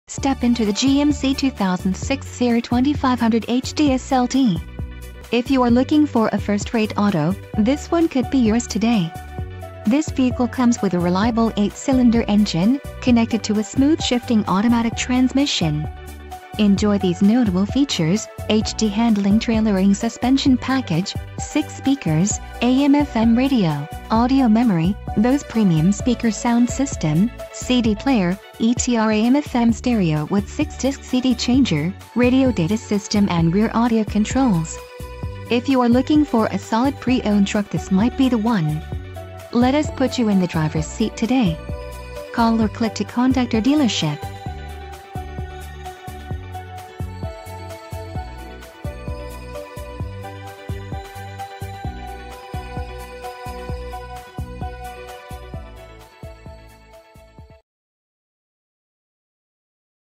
2006 GMC Sierra 2500HD SLT
Recent Arrival! LEATHER, 16 x 6.5 Polished Forged Aluminum Wheels, ABS brakes, Automatic Dual-Zone Climate Control, Black Power Folding Outside Rear-View Mirrors, Bose Premium Speaker Sound System, Compass, HD Handling/Trailering Suspension Package, Heated door mirrors, Heated front seats, Illuminated entry, Memory seat, Remote keyless entry, Ultrasoft Leather Seat Trim. 4WD Allison 1000 6-Speed Automatic Duramax 6.6L V8 TurbodieselNobody beats a Veterans Ford deal!!